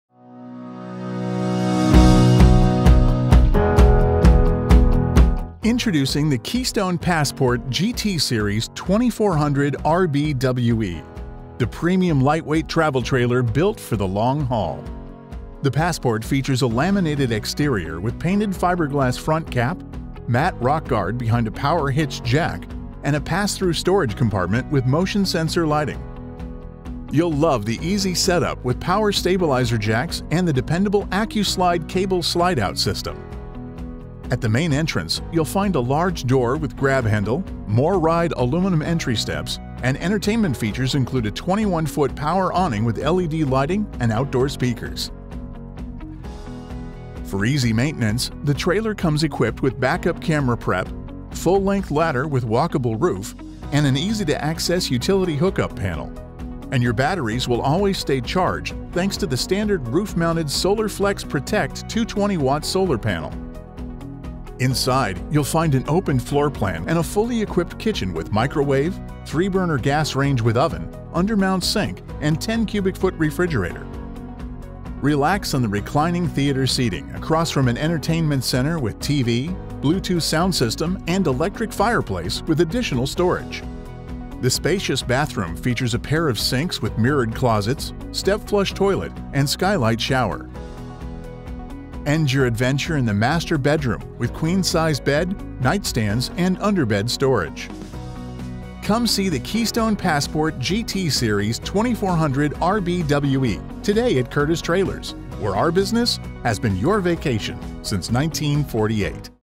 Passport 2400RBWE, the perfect light weight travel trailer!
Introducing the Keystone Passport GT Series 2400RBWE, the premium light weight travel trailer built for the long haul.

The Passport features a laminated exterior with painted fiberglass front cap, matte rock guard behind a power hitch jack, and a pass-through storage compartment with motion sensor lighting.

You’ll love the easy set up with power stabilizer jacks and the dependable accu-slide cable slideout system.

At the main entrance you’ll find a large door with grab handle, MORRYDE aluminum entry steps and entertainment features include a 21’ power awning with LED lighting and outdoor speakers.

For easy maintenance, the trailer comes equipped with backup camera prep, full-length ladder with walkable roof, and an easy to access utility hook up panel. And your batteries will always stay charged thanks to the standard roof mounted Solar Flex Protect 220 watt solar panel.

Inside, you’ll find an open floorplan with attractive vinyl flooring and a fully equipped kitchen with microwave, 3 burner gas range with oven, undermount sink and 10 cubic foot refrigerator.

Relax on reclining theater seating across from an entertainment center with TV, Bluetooth Sound System and electric fireplace with additional storage.

The spacious bathroom features a pair of sinks with mirrored closets, step flush toilet, and skylight shower.

End your adventure in the master bedroom with Queen size bed, night stands, and underbed storage.

Come see the Keystone Passport GT Series 2400RBWE today at Curtis Trailers, where our business has been your vacation since 1948.